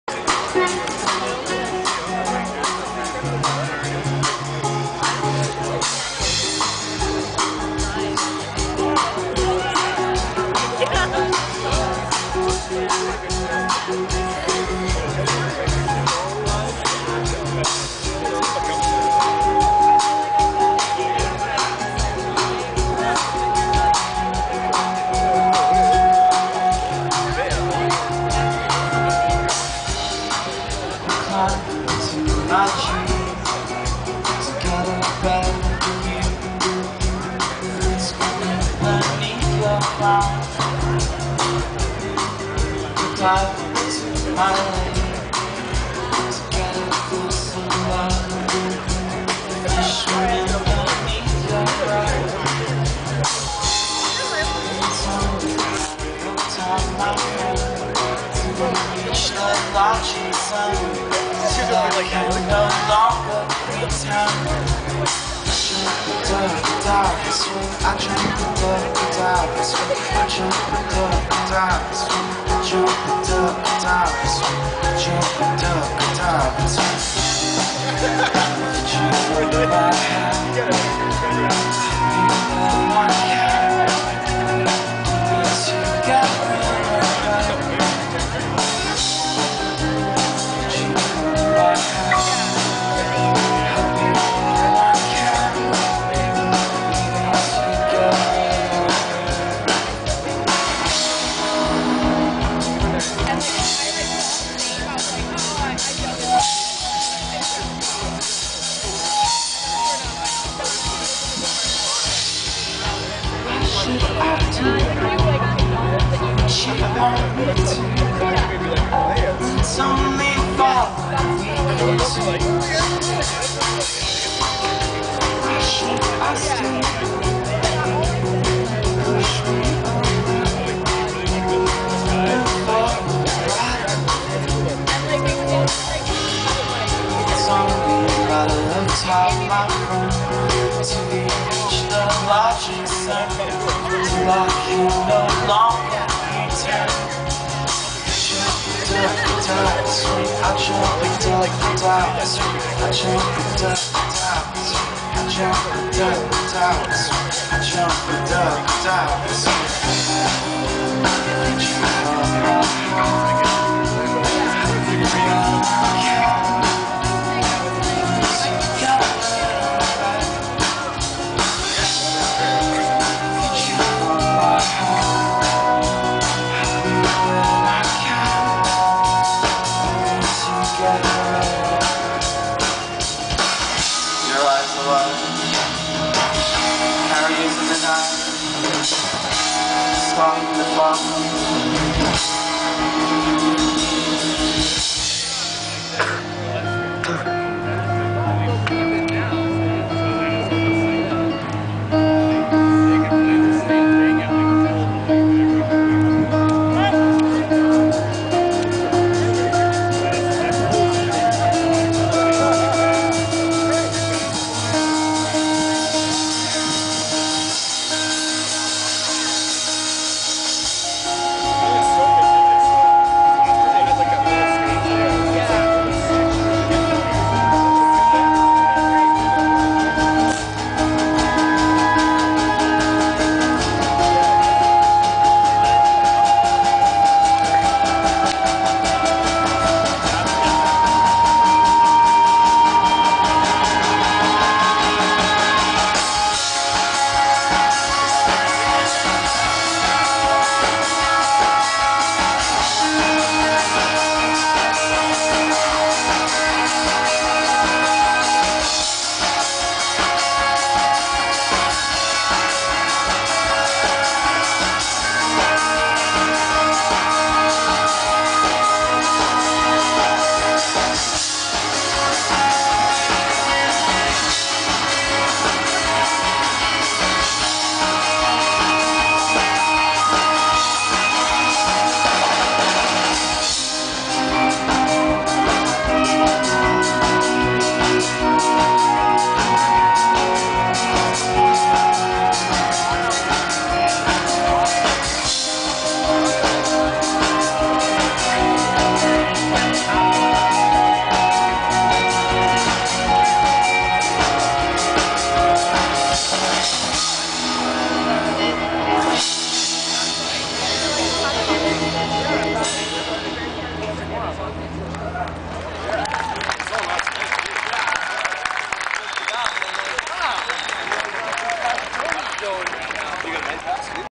Brasstronaut @ Royal Athletic Park (Rifflandia 2012)
brasstronaut playing at rifflandia.